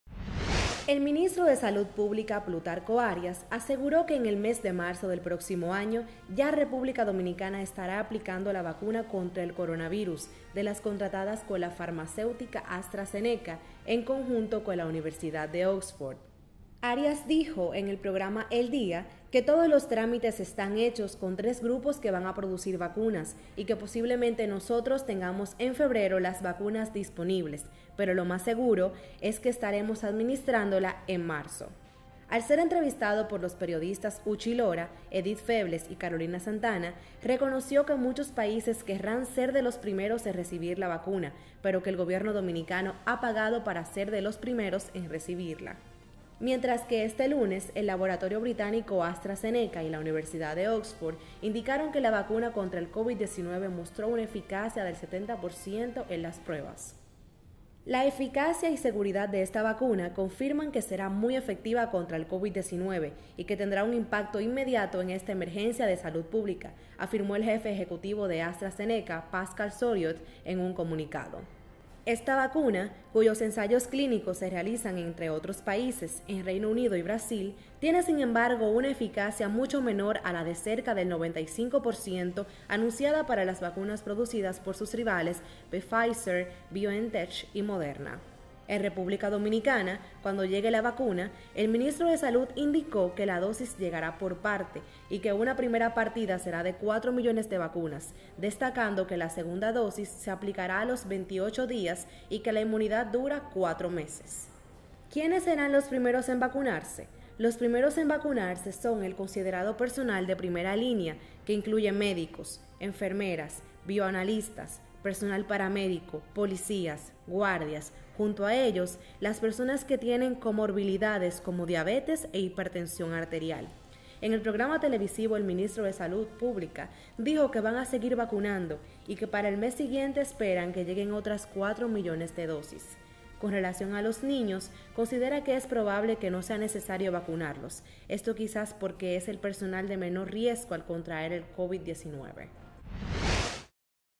En marzo en RD comenzarán aplicar 4 millones de vacunas contra el coronavirus
El ministro de Salud Pública, Plutarco Arias, aseguró que en el mes de marzo del próximo año ya República Dominicana estará aplicando, la vacuna contra el coronavirus, de las contratadas con  la farmacéutica AstraZeneca, en conjunto con la Universidad de Oxford.

Arias dijo, en el programa El Día (Telesistema 11), que todos los trámites están hechos con tres grupos que van a producir vacunas y  que “posiblemente nosotros tengamos en febrero las vacunas disponibles, pero lo más seguro, seguro es que estaremos administrándola en marzo”.